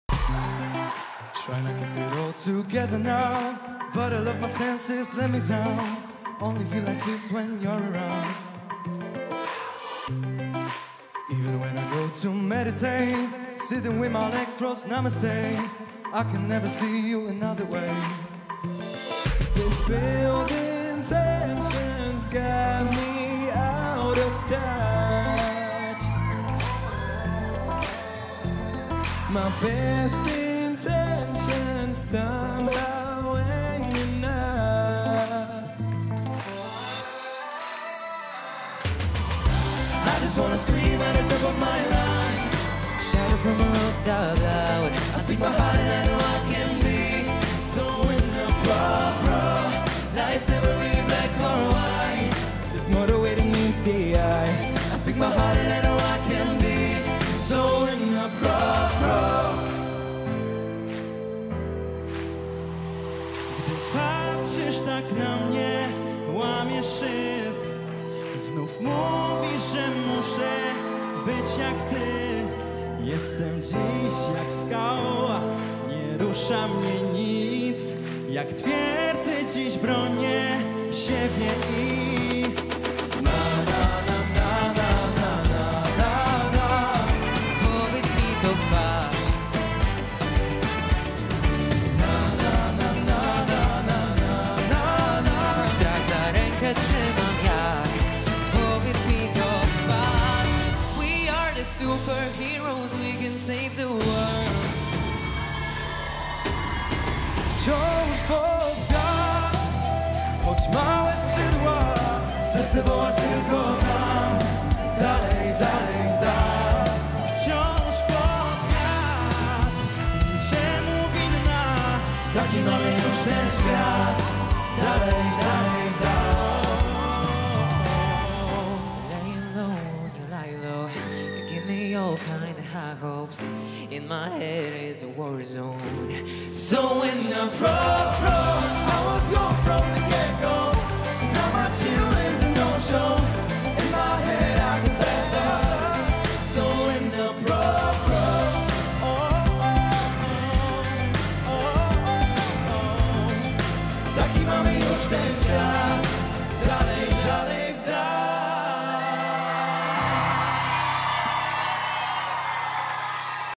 4Dreamers – piosenki gwiazd „The Voice Kids”
Uwielbiany przez publiczność boys band na macierzystej scenie. Jakub Szmajkowski, Mateusz Gędek, Maksymilian Więckowski i Tomasz Gregorczyk wzięli udział w pierwszej edycji programu.